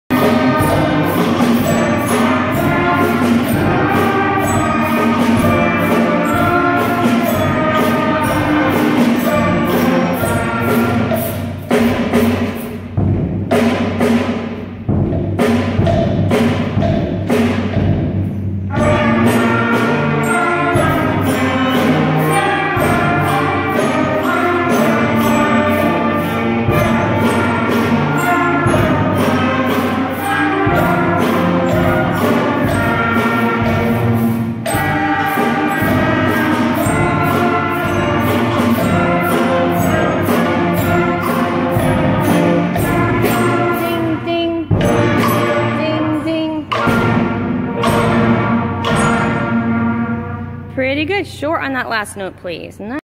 New band members 11/7/18 (read description under video)
Silver Band joins us for the 1st time. Productive rehearsal with limited talking thank you!! I have noted who is missing music and will be emailing parts. Drummers I will write more parts for you to play!

Merritt School and Edward H Bryan School 4th/5th